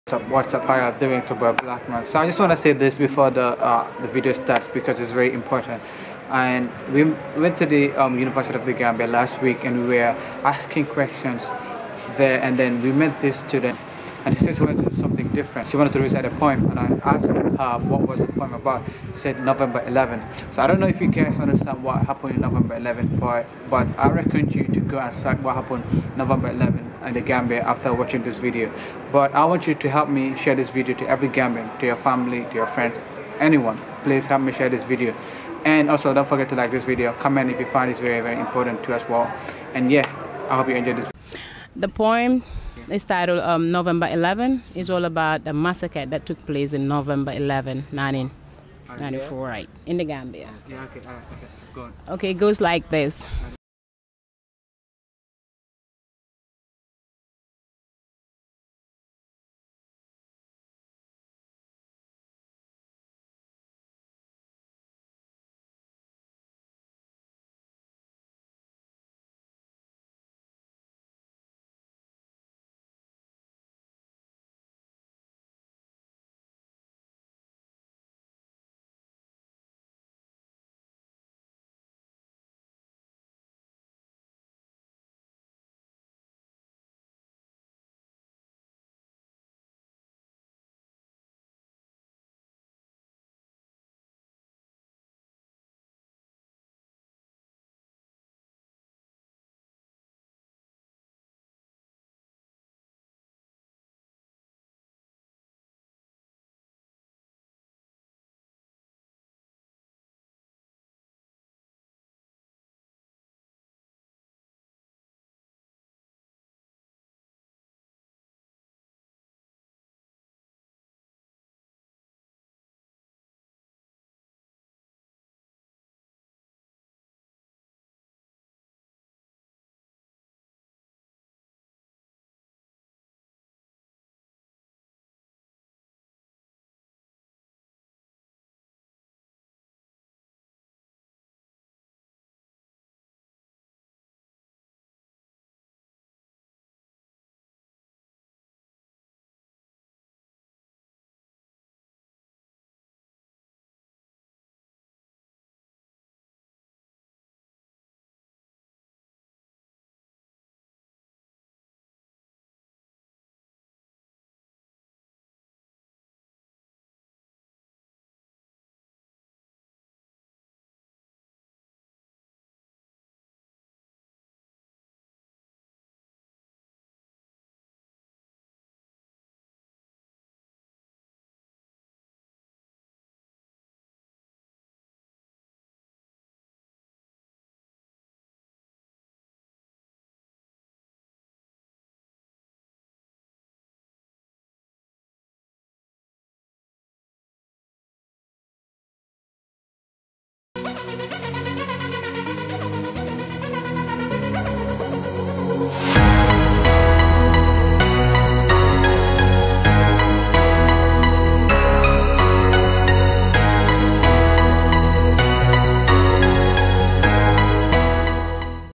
The Gambia.. November 11.. *Must Watch*
Here's a poem about November 11, 1994 in The Gambia.